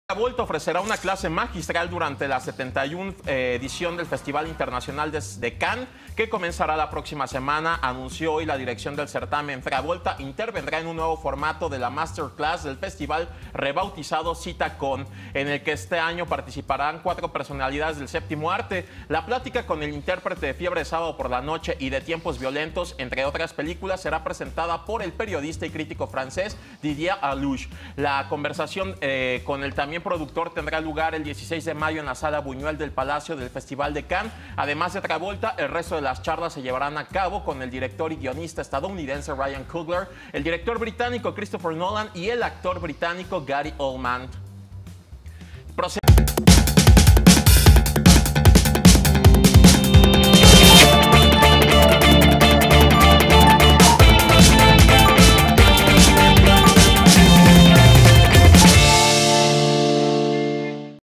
John Travolta dará Charla Magistral en la edición 71 de Cannes
John Travolta participará con una Charla Magistral en la edición 71 del Festival de Cine de Cannes, en el nuevo formato llamado 'Cita Con'.

02 de mayo 2018